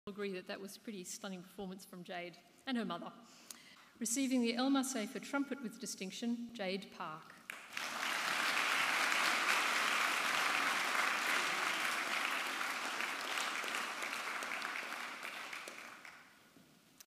2022 AMEB Diploma Award Ceremony
I was awarded my AMEB LMusA with Distinction in Trumpet at 2022 AMEB Diploma Award Ceremony